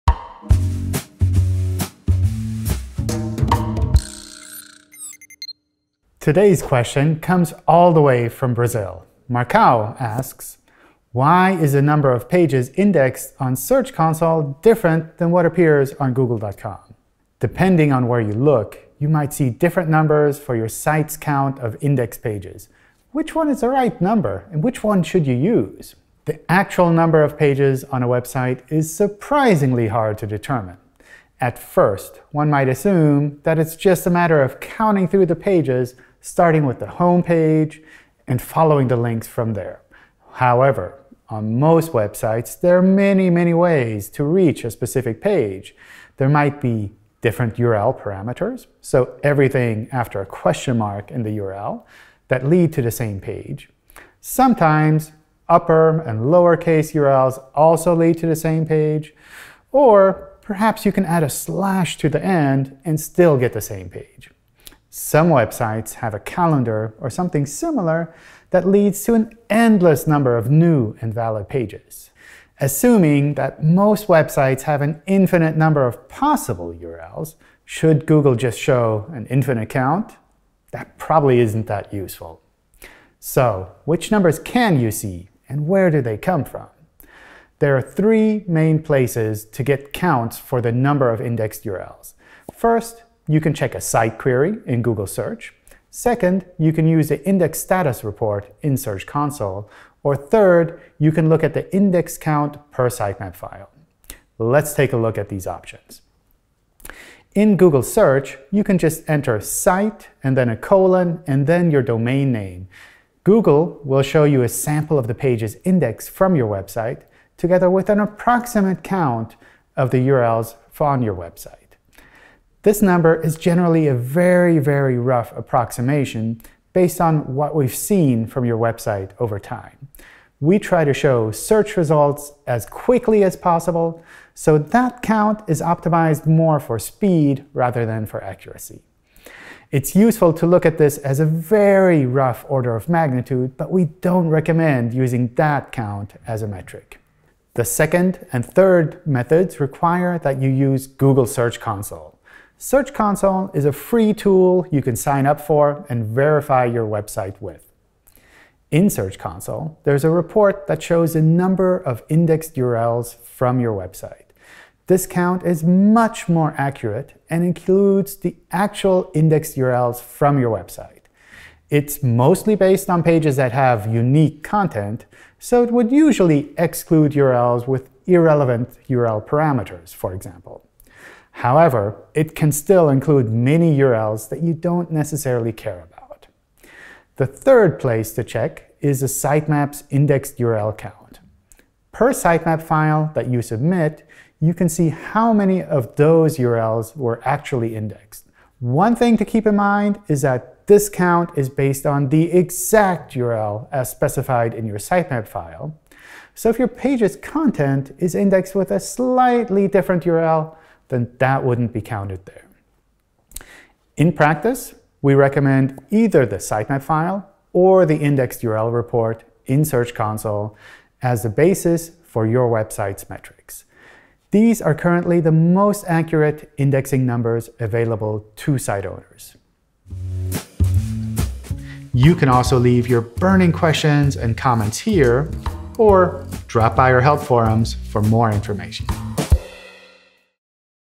Why does the indexed pages count vary?
Marcão Caloteiro from Brazil asked us “Why is the number of pages indexed on the Search Console different than what appears on Google.com?”

In this episode of SEO Snippets, we'll answer that question & provide some useful tips.

If you have a question, be sure to ask it in our Webmaster Help Forum